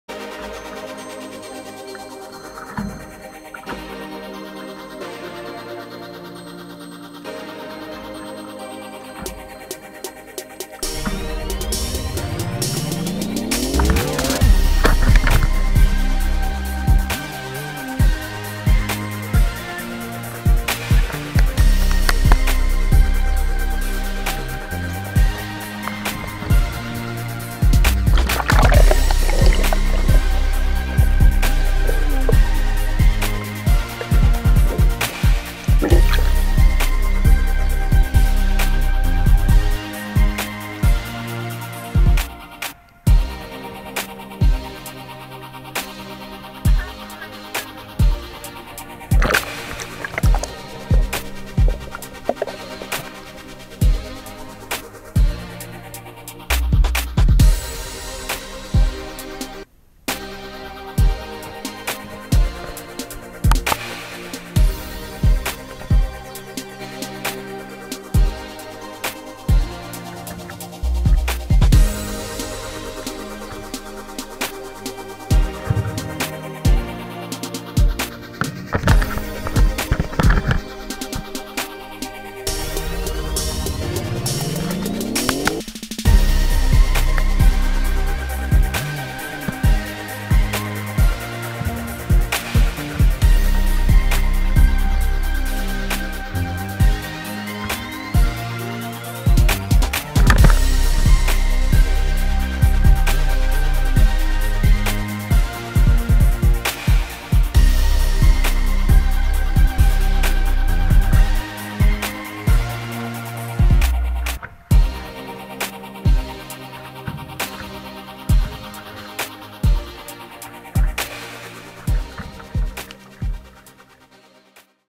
Paddle House SUP with Dolpins
SUP with Dolpins Paddle House, Redondo Beach, CA